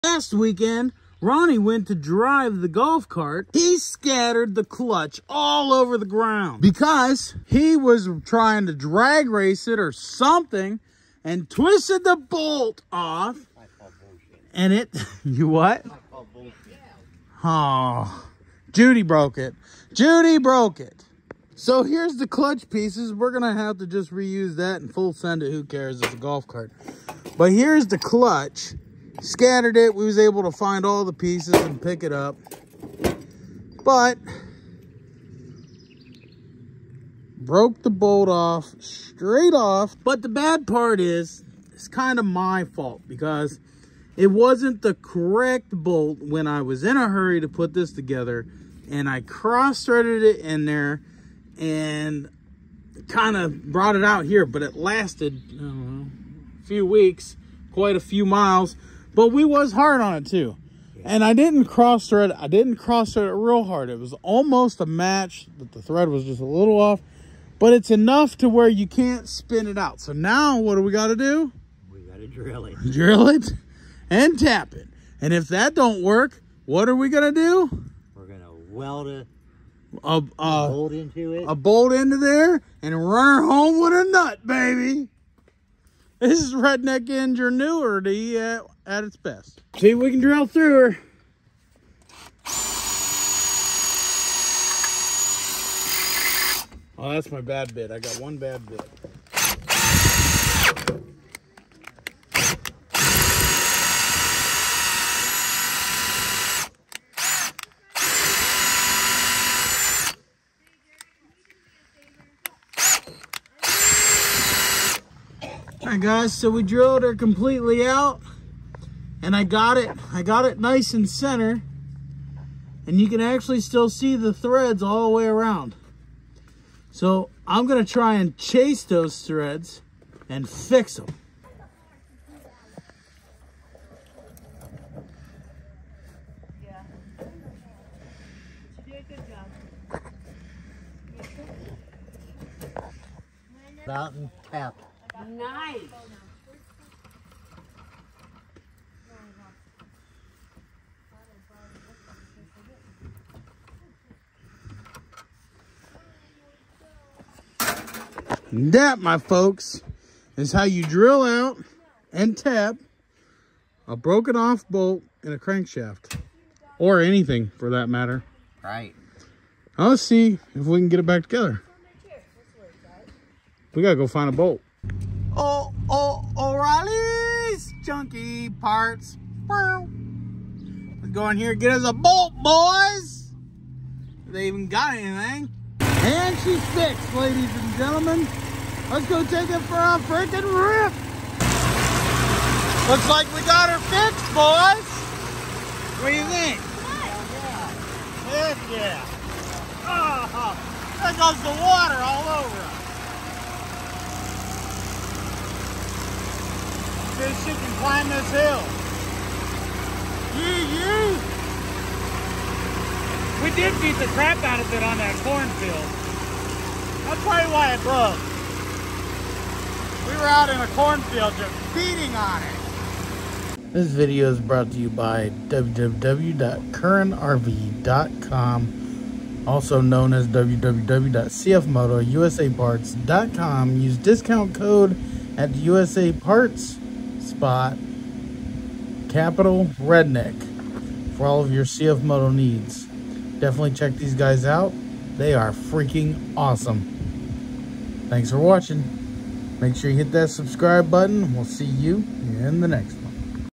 How to Remove Drill and Tap a Broken Bolt stuck in a crankshaft
The bolt holding the clutch on to our golf cart broke it wasn't a hardened steel bolt it was just a cheap one witch made drilling fairly easy. we chase the threads with a tap and go buy a better grade 8 bolt to use. we get the cart back up and moving pretty quickly.

Thank you for all of your support we hope to keep bringing you more content and we hope to get better and better.
Here is how you can support us.